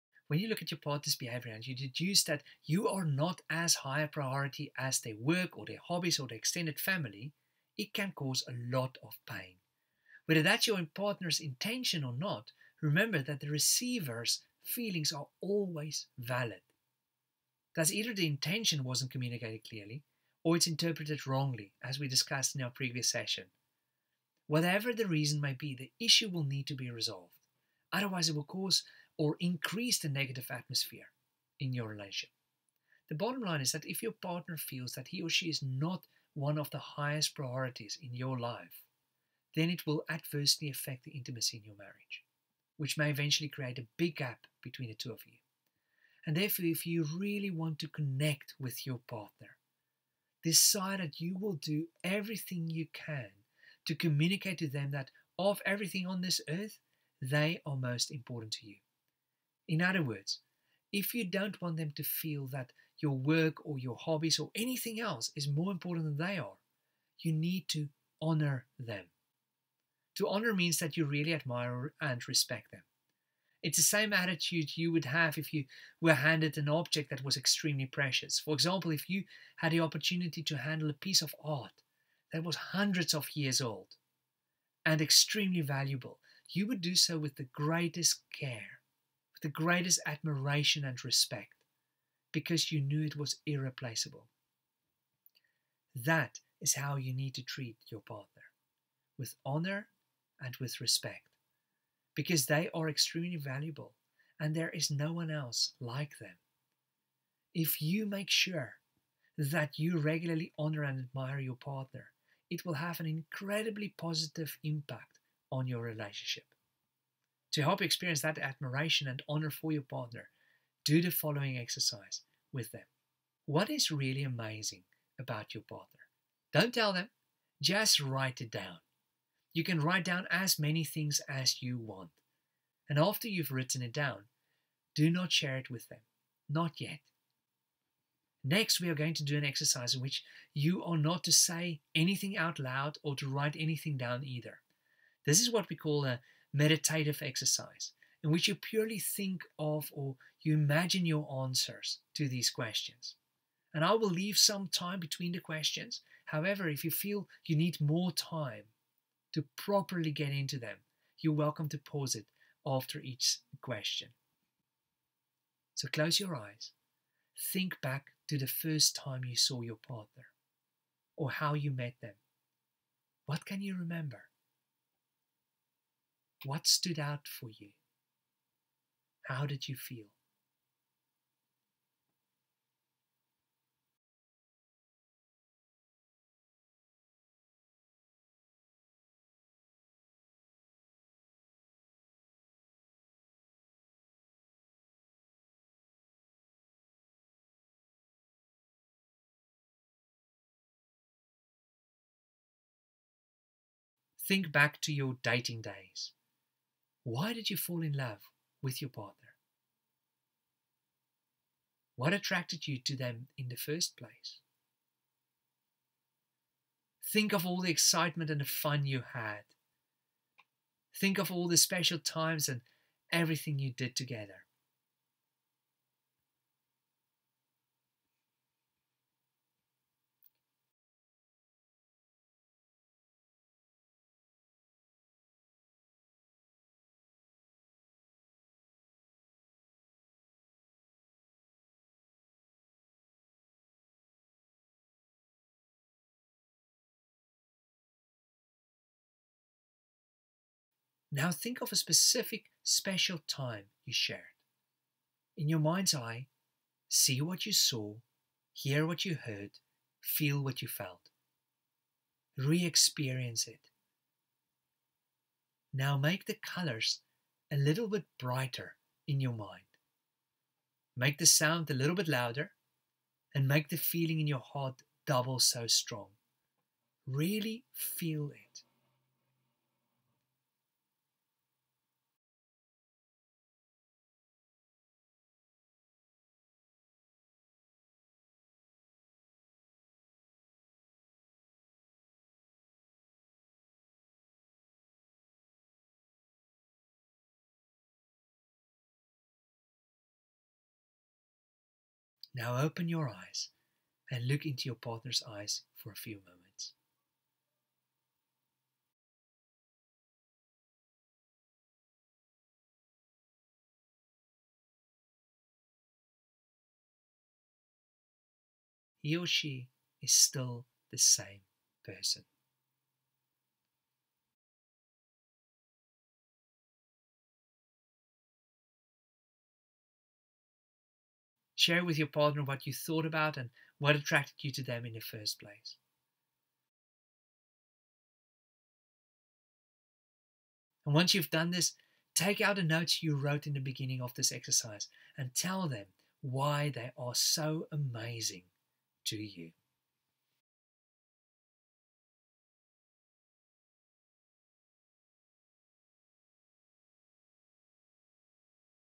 8. Connection
If you really want to connect with your partner, learn to truly honour them.